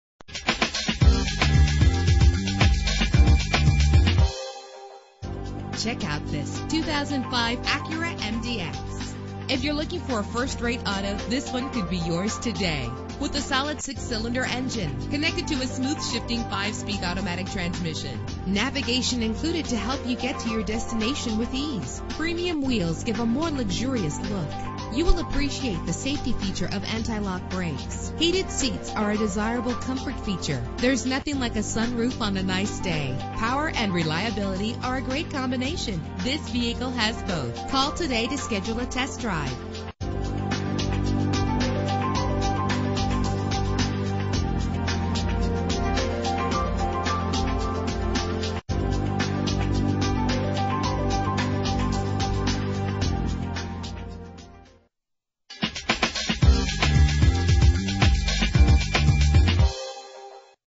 2005 Acura MDX - Denver CO
Year: 2005
Make: Acura
Model: MDX
Trim: 3.5 Touring w/ RES/ Navigation
Engine: 3.5 liter 6 cylinder 24 valve
Transmission: 5-Speed Automatic
Color: Steel Blue Metallic
Mileage: